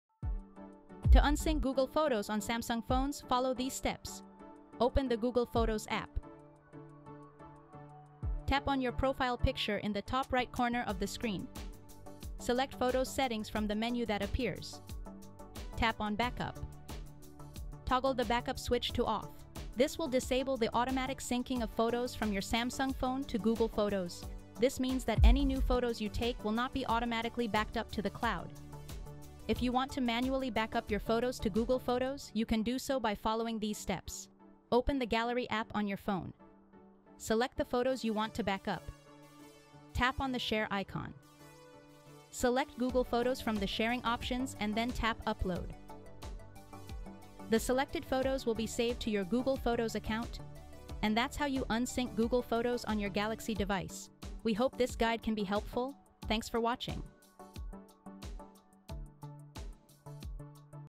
How To Unsync Google Photos on Samsung Galaxy Phones
Unsyncing Google Photos on your Samsung phone is a breeze with these steps: First, open the Google Photos app, tap your profile picture, select "Photo Settings," and then tap on "Backup." Simply switch the backup toggle off to halt automatic syncing, preventing your Samsung photos from being uploaded to Google Photos. If you prefer manual backups, open the Gallery app, select the photos, tap the share icon, choose Google Photos, and hit upload. Now, your Galaxy device won't automatically sync, giving you control over your photo backups.